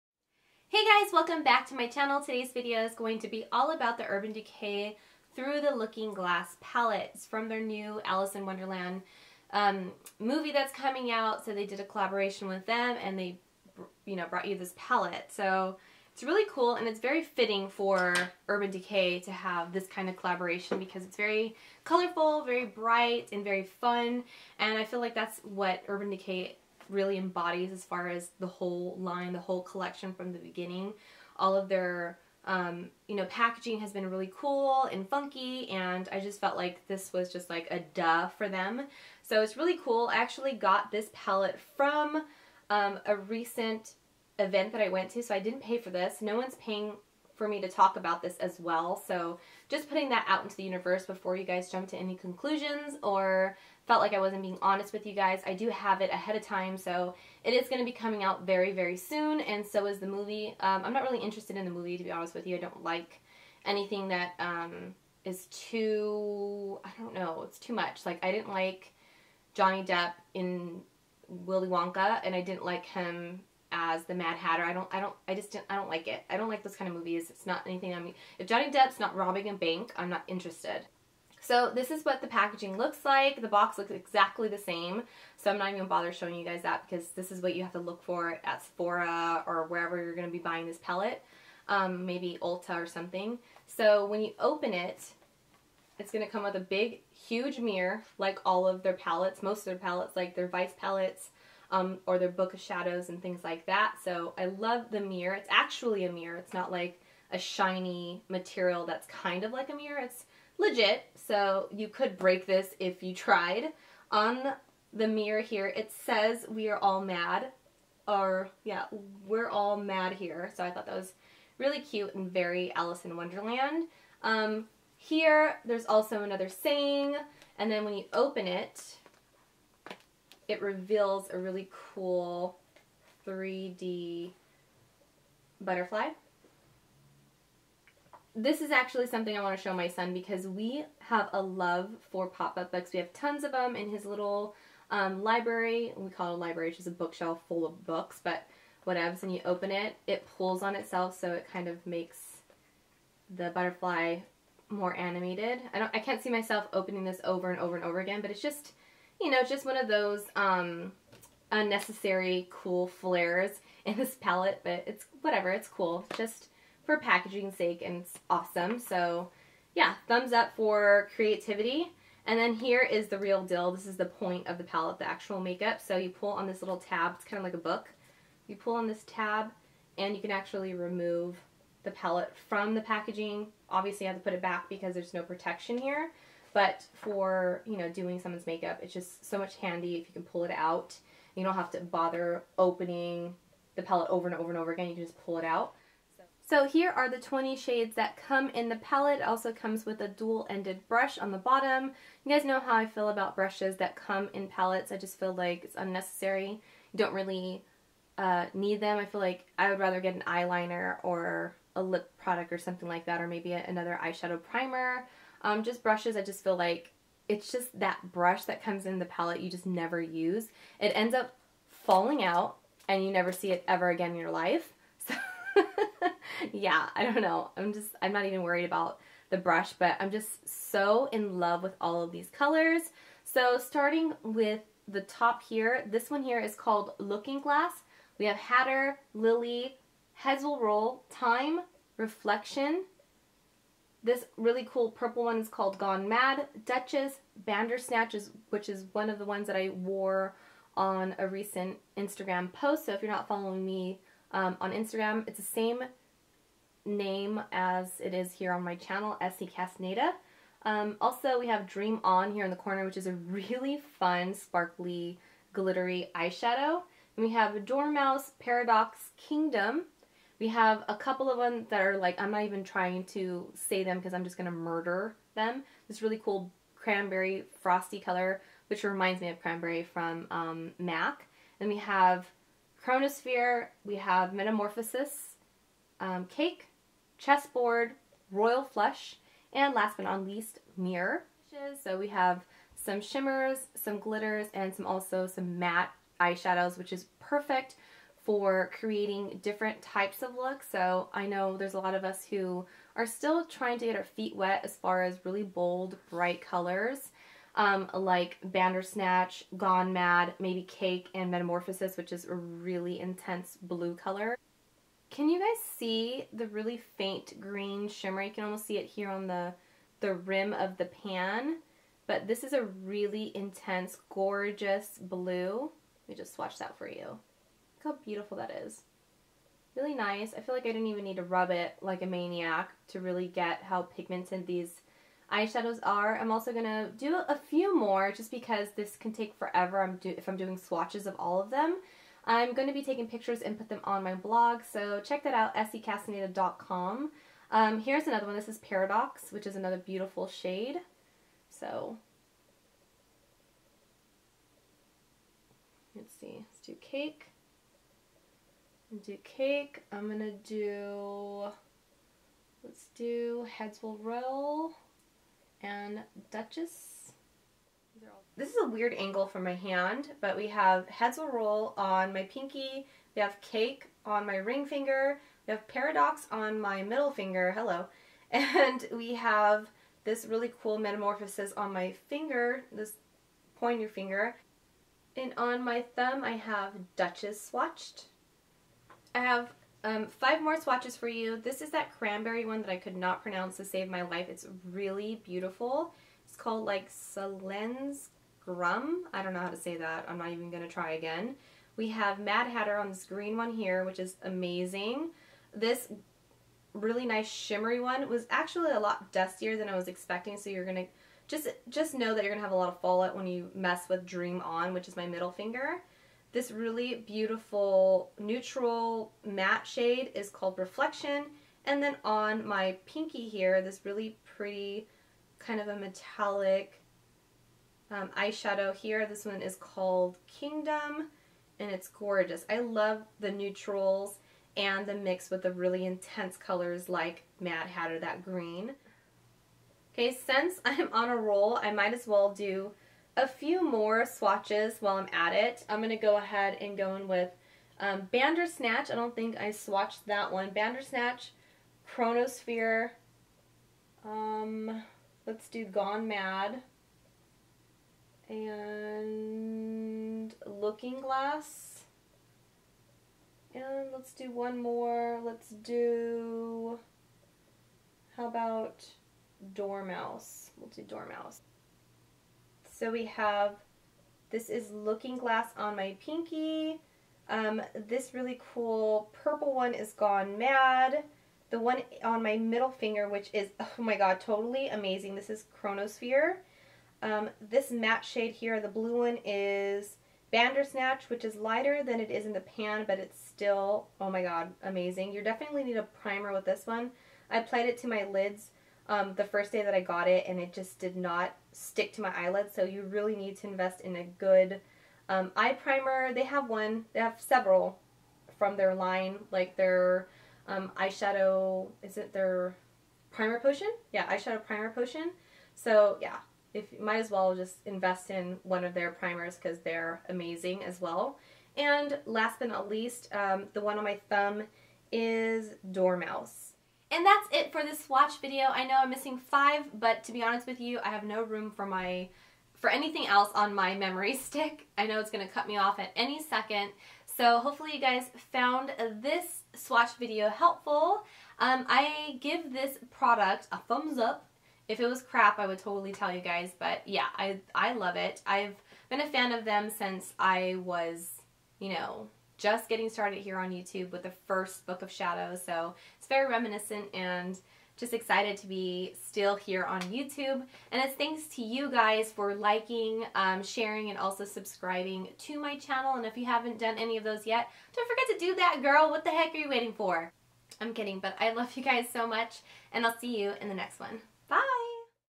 URBAN DECAY ALICE THROUGH THE LOOKING GLASS PALETTE | REVIEW + SWATCHES | SCCASTANEDA
♡ SNAPCHAT ♡: sccastaneda

Today is going to be a first impressions, review, and swatches of the new Urban Decay Through the Looking Glass Eyeshadow Palette. I got this little baby at an event recently but this is 100% all of my own opinions!